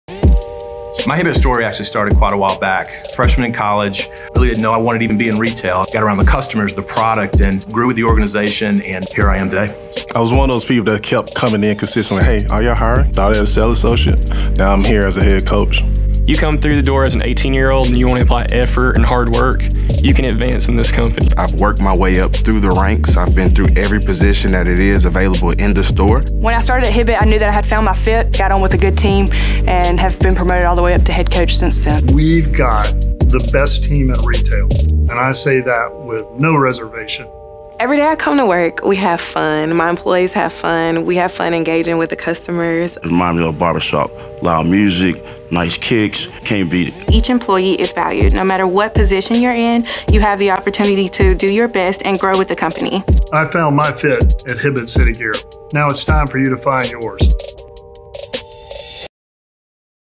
We're Hiring! Find Your Fit At Hibbett | City Gear
If you have an ambitious attitude, seek a fun and engaging work environment and have a passion for fashion, footwear and helping others find their perfect fits, then a job with Hibbett | City Gear is right for you! Find your fit at Hibbett | City Gear -- see our openings near